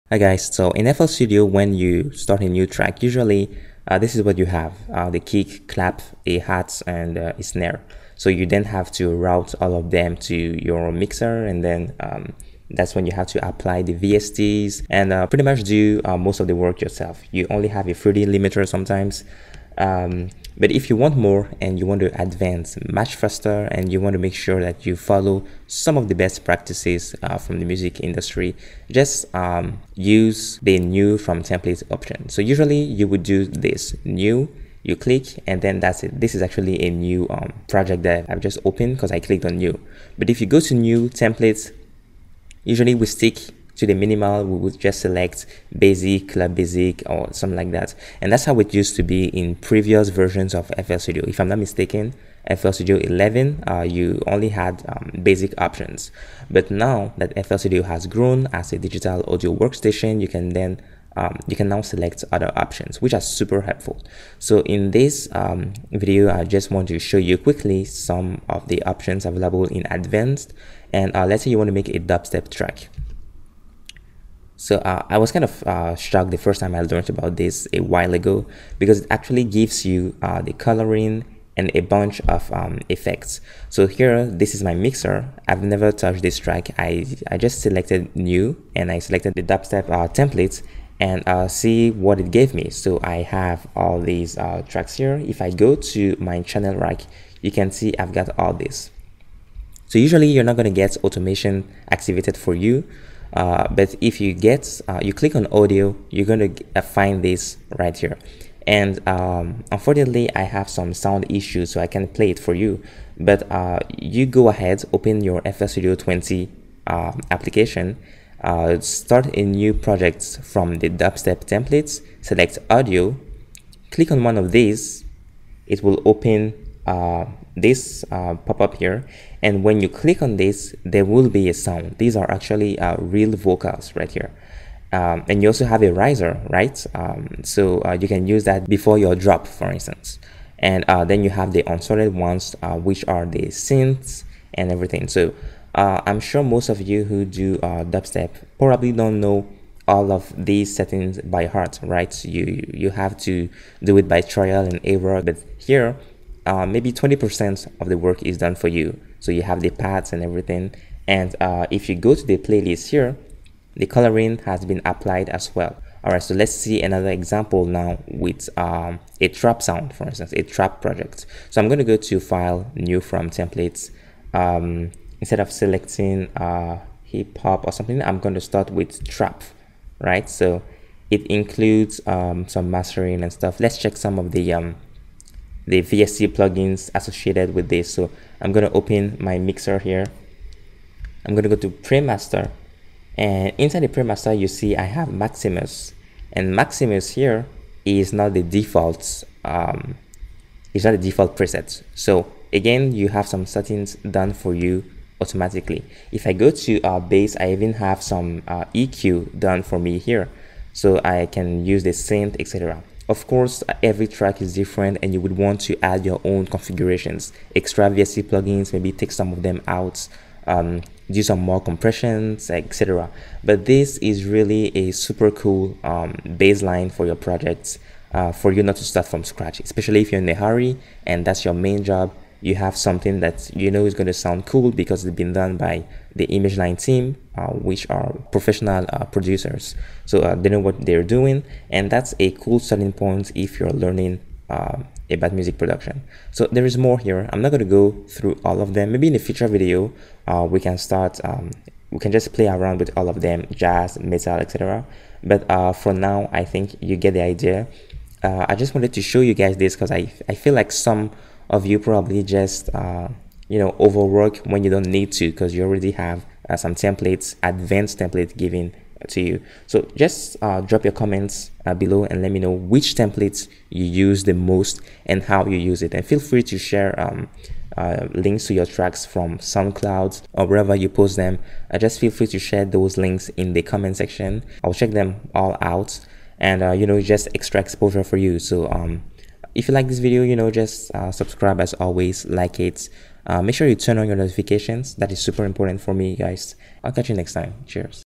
FL Studio 20 tutorial: Free templates including VST plugins and vocals
Have you ever used FL Studio 20's advanced templates for Dubstep, Trap or other project? In this quick tutorial for beginners, we look at how free templates in FL Studio 20 can help you speed up your music production by making use of already set up VST plugins, gaining access to vocal chops and not having to worry about routing tracks to your mixer or coloring in your playlist.